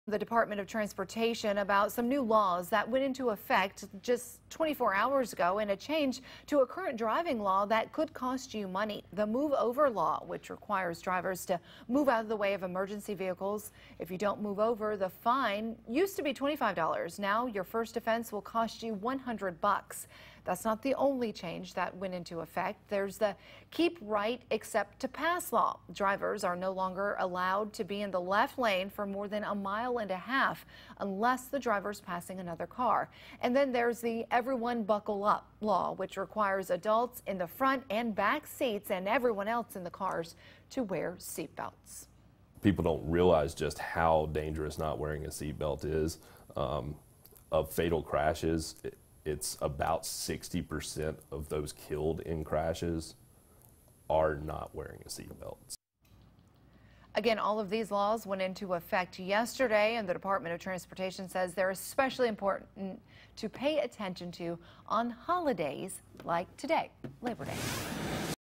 Aldot on New Driving Laws
We are hearing from the Department of Transportation about some new laws that went into effect over the weekend,